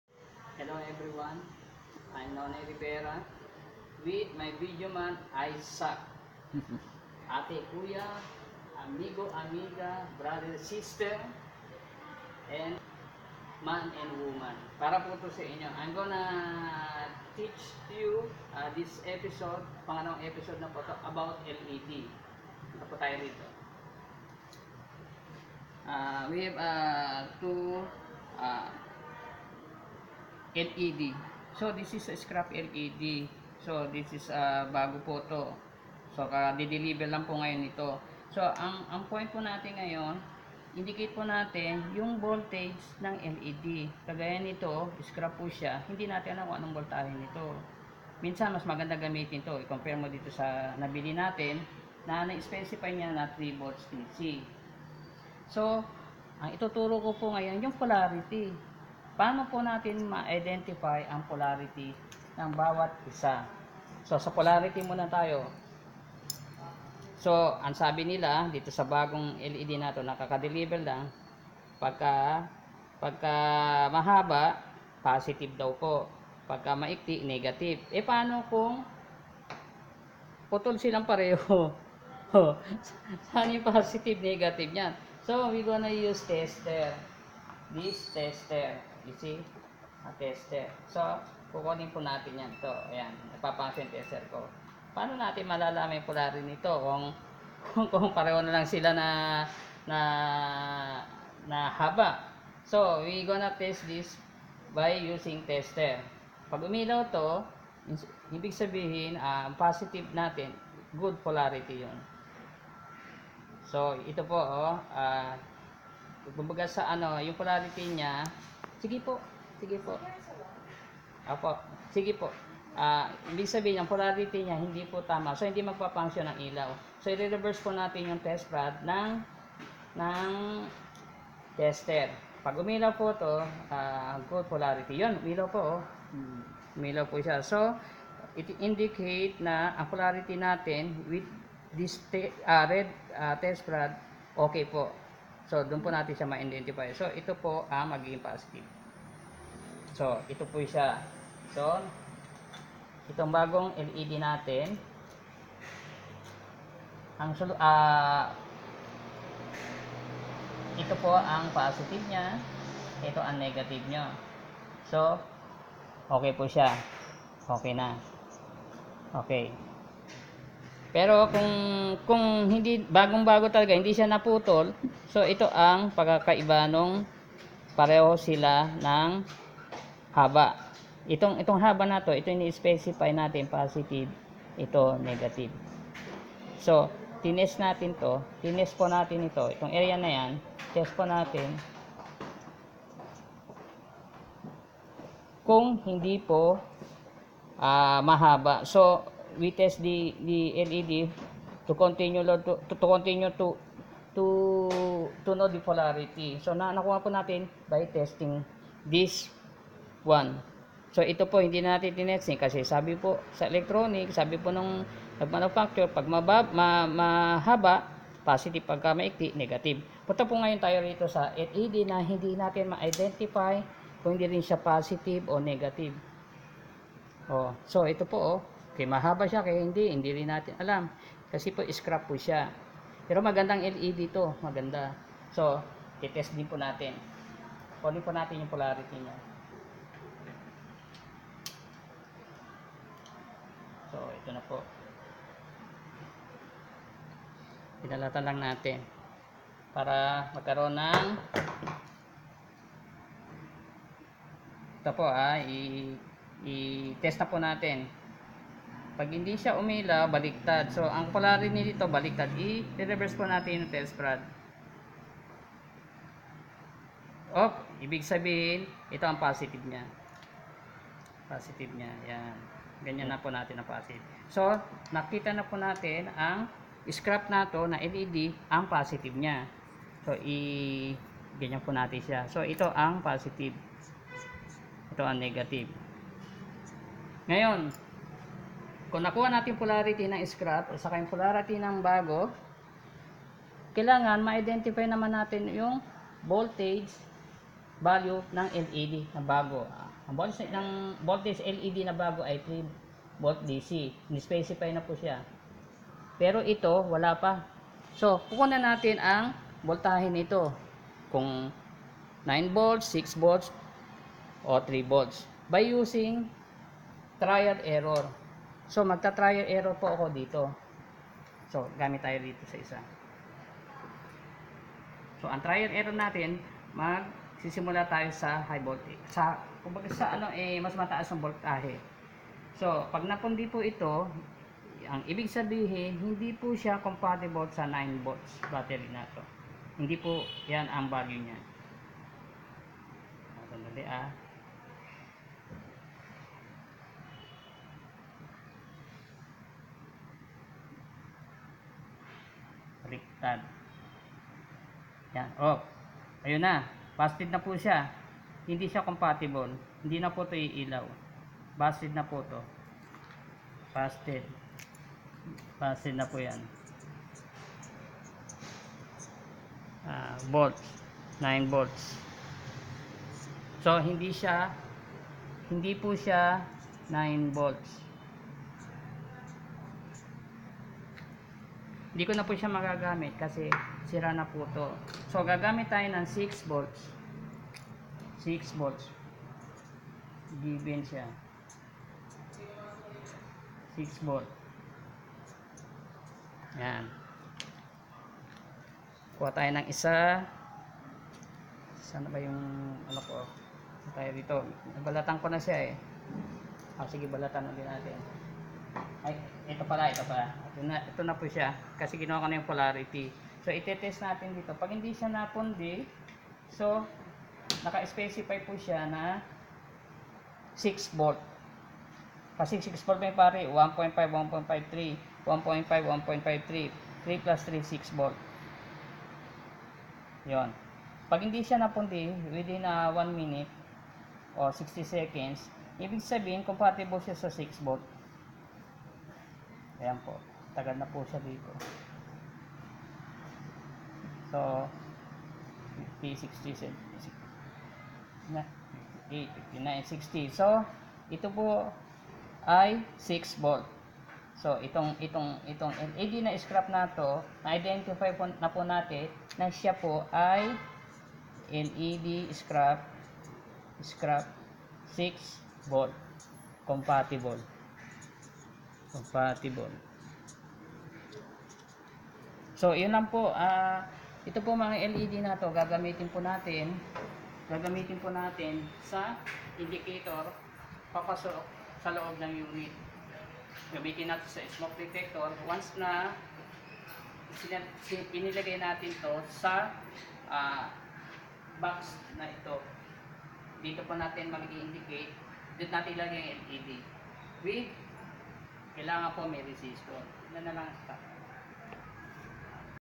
How to test LED light with voltage measurement and understanding LED polarity.
this is episode -2 actual  measurement and understanding  led polarity. using  tester.